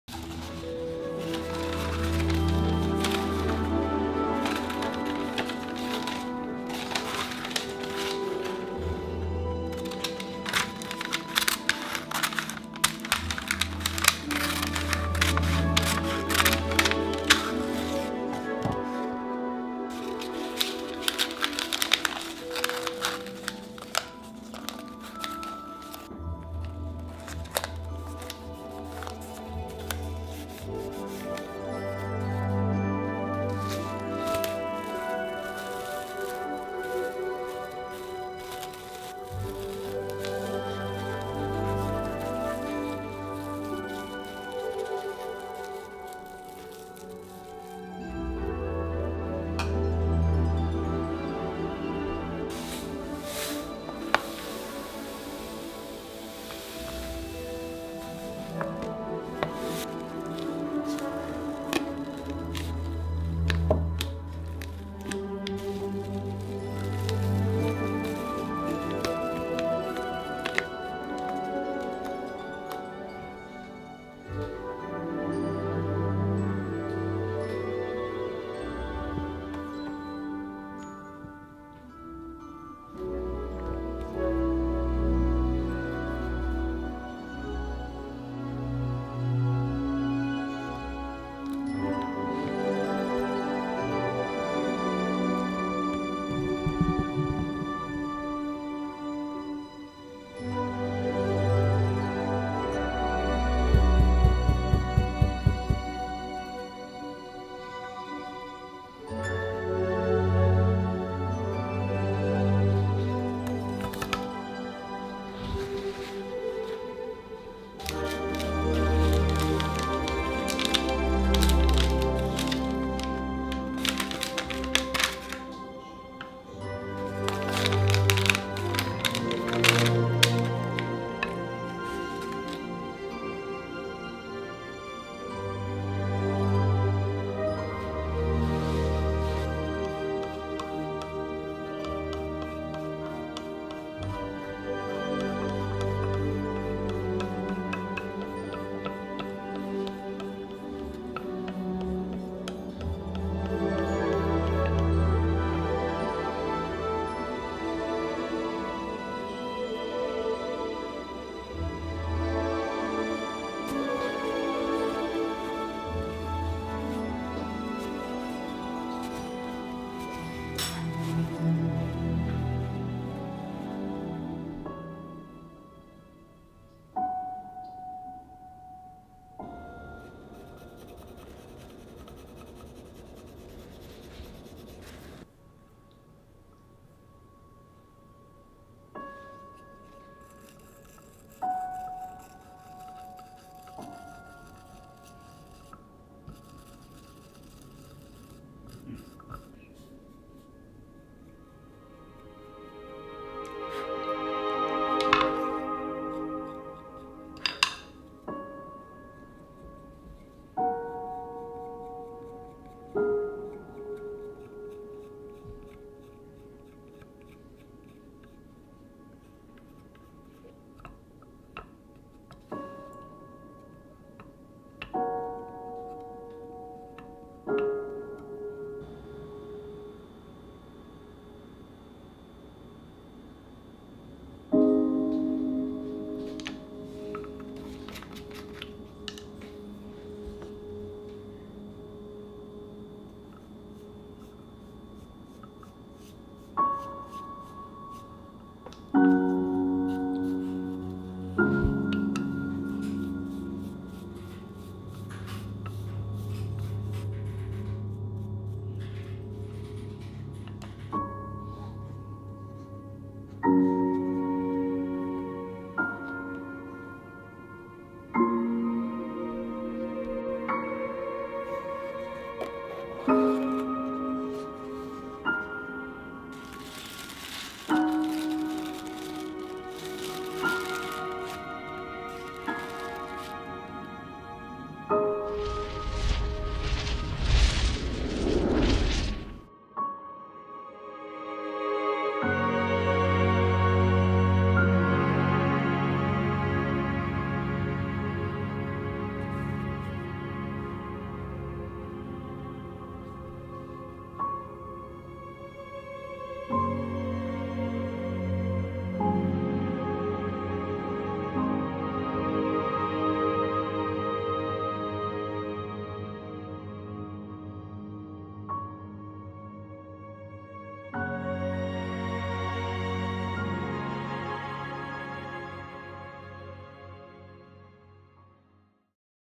DIY Harry Potter Chocolate Wand 해리포터 마법지팡이 초코릿으로 만들기
Hi, guys!
I've been making youtube baking videos over 3 years and lately I felt like stepping out of my comfort zone. So I decided to try new things out.
And here's my first attempt, Harry Potter chocolate wand!
I wasn't sure if I could make this but I just told myself not to be afraid of failure and tried it.
Because that's how we grow, right?
Anyway I'm pretty satisfied with my video overall.
I should definitely try more things I've never done :)

And Lastly, I really appreciate your all warm comments.
It keeps me going! THANK YOU SO MUCH!